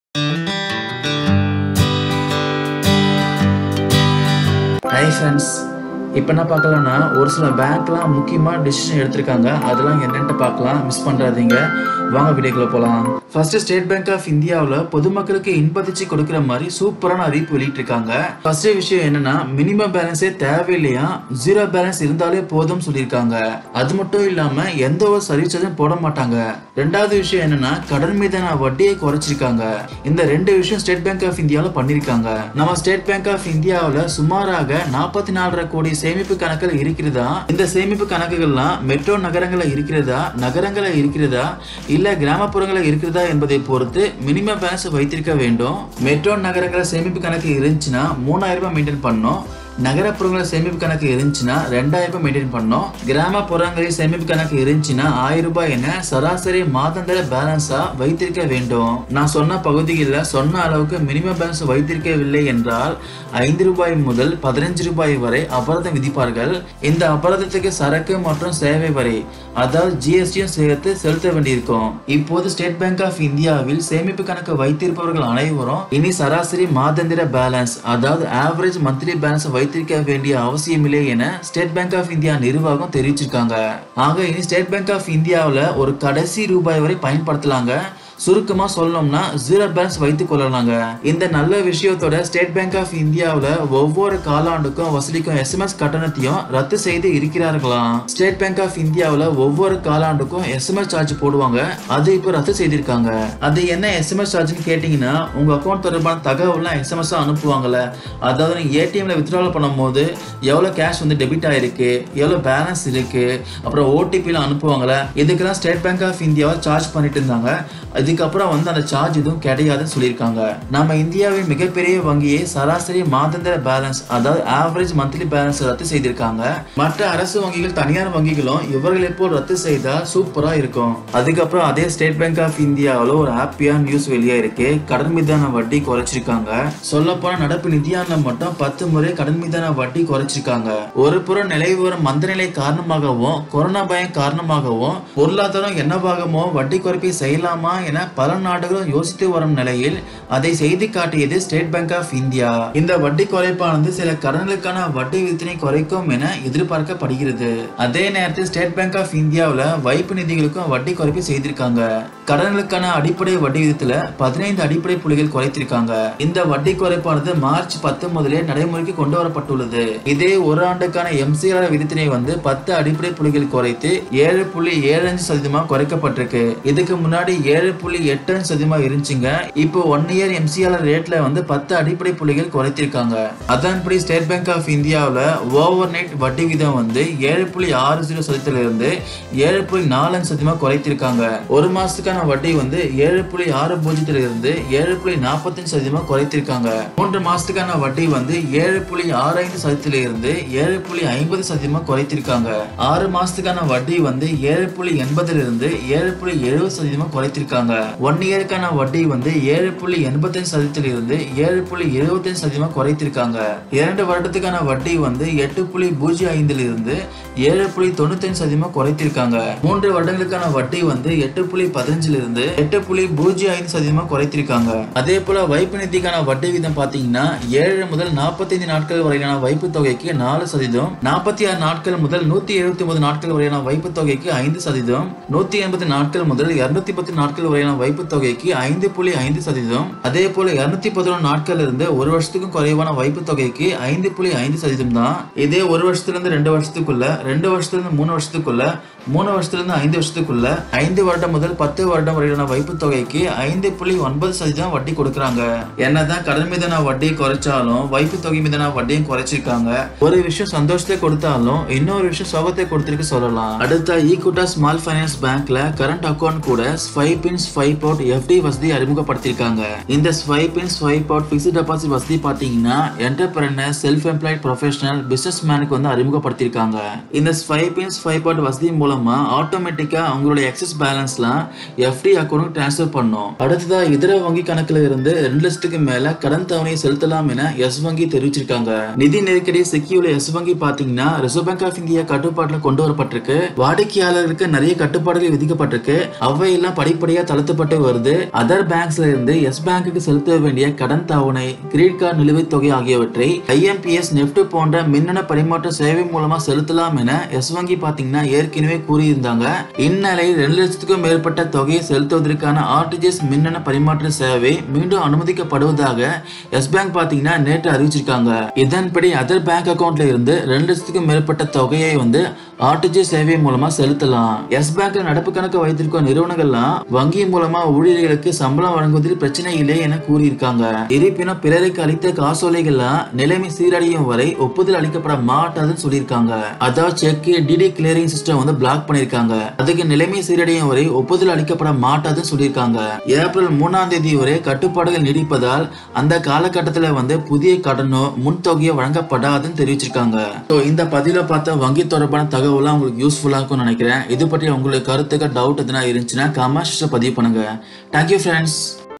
வங்கிகளின் அதிரடி! 45 கோடி பொதுமக்களுக்கு அடித்தது பாரு அதிர்ஷ்டம்! இதுவரை யாரும் வழங்காத சலுகை
வங்கிகளின் அதிரடி அறிவிப்பு! 45 கோடி பொதுமக்களுக்கு அடித்தது பாரு அதிர்ஷ்டம்! இதுவரை யாரும் வழங்காத சலுகை! இனி உங்கள் பணத்துக்கு கட்டுப்பாடு இல்லை

1. எஸ்பிஐ வாடிக்கையாளரா நீங்க.. உங்களுக்கு ஒரு ஹேப்பி நியூஸ்.. இதோ..!

எஸ்பிஐ அவ்வப்போது தனது வாடிக்கையாளர்களை கவரும் விதமாக வட்டி குறைப்பை அறிவித்து வருகிறது. சொல்லப்போனால் நடப்பு நிதியாண்டில் இது 10 முறையாகும்.

எனினும் அதே நேரத்தில் வங்கி வைப்பு நிதிகளுக்கும் வட்டி குறைப்பு செய்யப்பட்டுள்ளது. கடன்களுக்கான அடிப்படை வட்டி விகிதத்தில் 15 அடிப்படை புள்ளிகள் வரை குறைத்துள்ளது. இந்த வட்டி குறைப்பானது மார்ச் 10 முதலே நடைமுறைக்கு கொண்டு வரப்பட்டுள்ளதாகவும் கூறப்படுகிறது. இதே ஓராண்டு எம்சிஎல்ஆர் விகிதத்தினை 10 அடிப்படை புள்ளிகள் குறைத்து 7.75% ஆக குறைத்துள்ளது. இது முன்பு 7.85% ஆக இருந்தது குறிப்பிடத்தக்கது.

2. எஸ்பிஐ-ல் இனி மினிமம் பேலன்ஸ் பிரச்சனை இல்லை..!

இப்போது, ஸ்டேட் பேங்க் ஆஃப் இந்தியாவில் சேமிப்பு கணக்கு வைத்திருப்பவர்கள் அனைவரும், இனி சராசரி மாதாந்திர பேலன்ஸ் (Average Monthly Balance) வைத்திருக்க வேண்டிய அவசியம் இல்லை எனச் சொல்லி இருக்கிறது எஸ்பிஐ நிர்வாகம். எனவே இந்த செய்தி எஸ்பிஐ-ல் வங்கிக் கணக்கு வைத்திருப்பவர்கள் வயிற்றில் பால் வார்த்து இருக்கிறது.

3. Equitas Small Finance Bank introduces Current Account with 'Sweep In Sweep Out' FD

The key features of Equitas Current Account with Sweep In Sweep Out FD are: Automatic transfer of excess balance into Sweep FD for 90 days Weekly sweep out frequency for FDs No Premature Penalty for Sweep In Sweep Out FD

4. இதர வங்கி கணக்குகளில் இருந்து ரூ.2 லட்சத்துக்கு மேல் கடன் தவணை செலுத்தலாம் - யெஸ் வங்கி

இதர வங்கிக்கணக்கில் இருந்து யெஸ் வங்கிக்கு செலுத்த வேண்டிய கடன் தவணை, கிரெடிட் கார்டு நிலுவைத்தொகை ஆகியவற்றை ஐ.எம்.பி.எஸ்., என்.இ.எப்.டி. போன்ற மின்னணு பணபரிமாற்ற சேவை மூலம் செலுத்தலாம் என்று யெஸ் வங்கி ஏற்கனவே கூறியிருந்தது.

இந்நிலையில், ரூ.2 லட்சத்துக்கு மேற்பட்ட தொகையை செலுத்துவதற்கான ஆர்.டி.ஜி.எஸ். மின்னணு பணபரிமாற்ற சேவை மீண்டும் அனுமதிக்கப்படுவதாக யெஸ் வங்கி நேற்று தெரிவித்தது. இதன்படி, இதர வங்கிக்கணக்கில் இருந்து ரூ.2 லட்சத்துக்கு மேற்பட்ட தொகையை ஆர்.டி.ஜி.எஸ். சேவை மூலம் செலுத்தலாம் என்று கூறியுள்ளது.